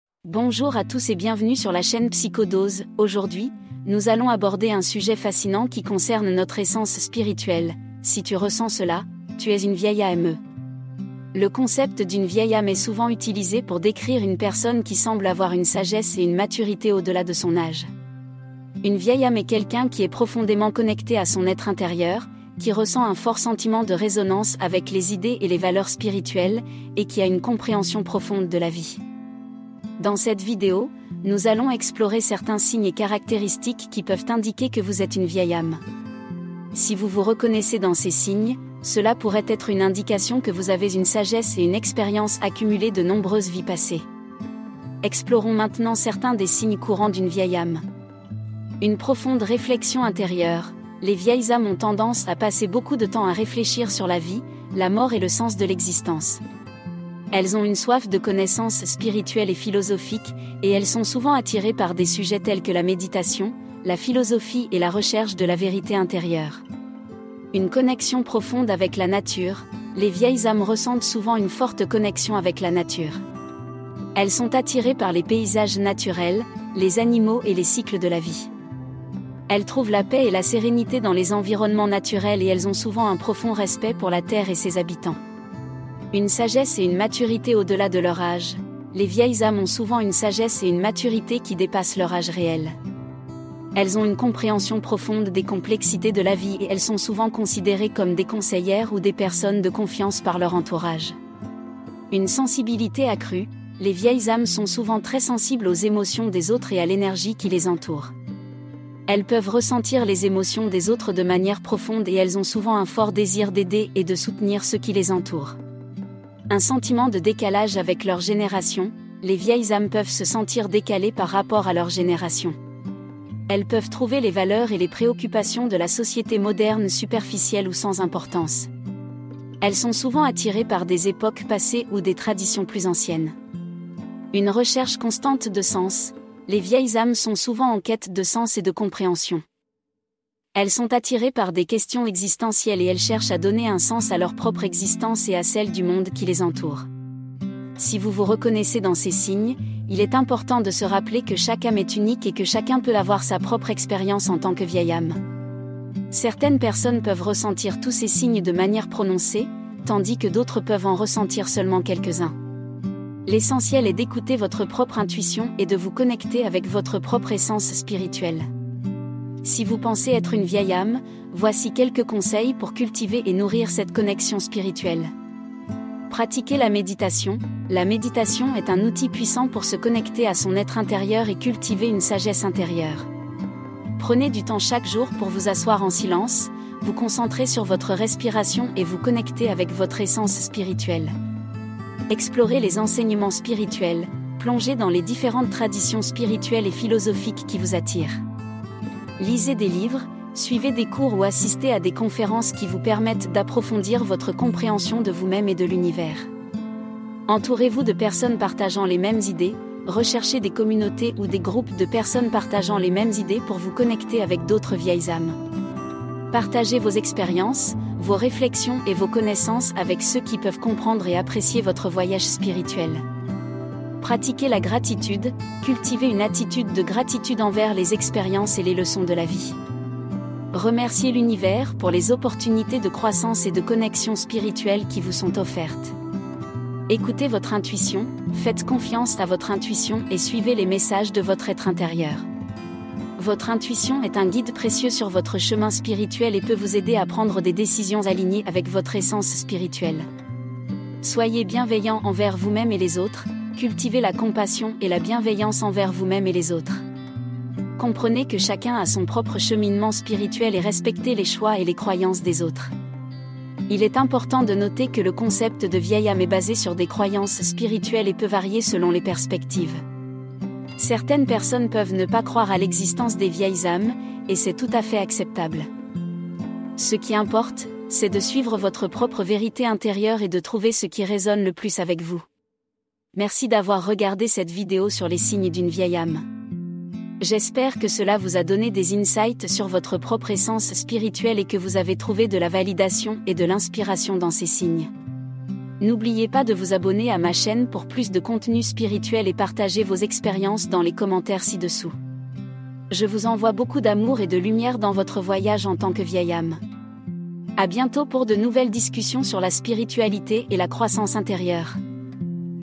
Si tu ressens cela, tu es une VIEILLE ÂME
Bonjour à tous et bienvenue sur la chaîne Psycho Dose !Aujourd'hui, nous allons aborder un sujet fascinant qui concerne notre essence spirituelle : "Si tu ressens cela, tu es une VIEILLE  ME".
Le concept d'une vieille âme est souvent utilisé pour décrire une personne qui semble avoir une sagesse et une maturité au-delà de son âge. Une vieille âme est quelqu'un qui est profondément connecté à son être intérieur, qui ressent un fort sentiment de résonance avec les idées et les valeurs spirituelles, et qui a une compréhension profonde de la vie.